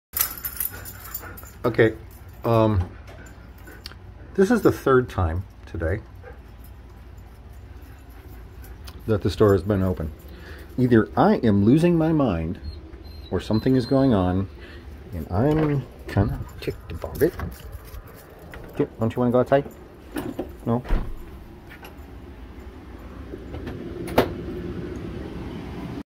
Third time today I've found the inner door open.

I open it like this when I let the dogs out. Or dog, because Piper likes to go outside all the time, but Isis usually wants to hang inside where it's warm, or have me go outside with and play.

The door doesn't open by itself. It takes a good push to get it closed, and it's not loose on its hinges. This is me forgetting to shut the inner door three# times today.

I'm gonna chalk this up to stress.